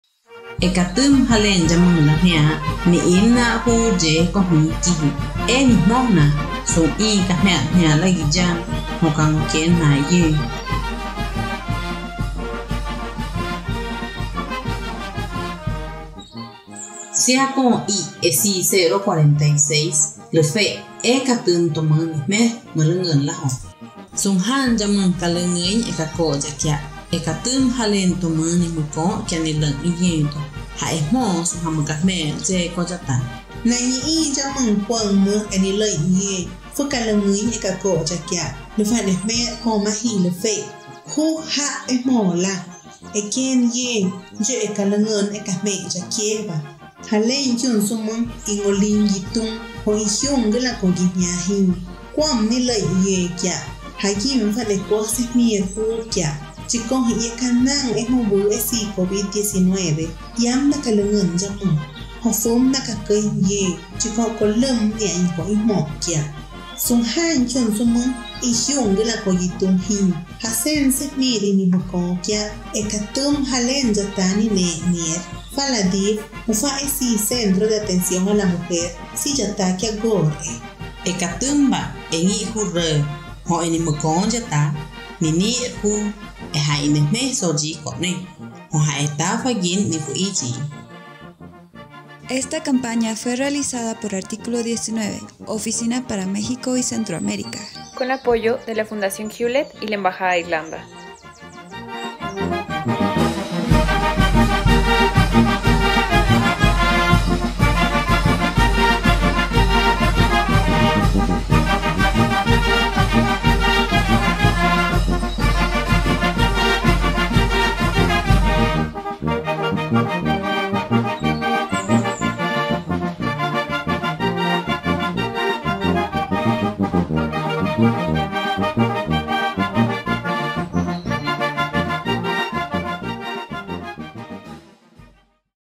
¿Qué hacer en caso de sufrir violencia sexual? (Chinanteco)
👩🏾 ⚠️Si has vivido violencia sexual, es importante que conozcas tu derecho a prevenir, o interrumpir un embarazo no deseado.

 En este vídeo, te explicamos cómo, conoce la NOM046 de la Secretaría de Salud :

🎧 Locutado en Chinanteco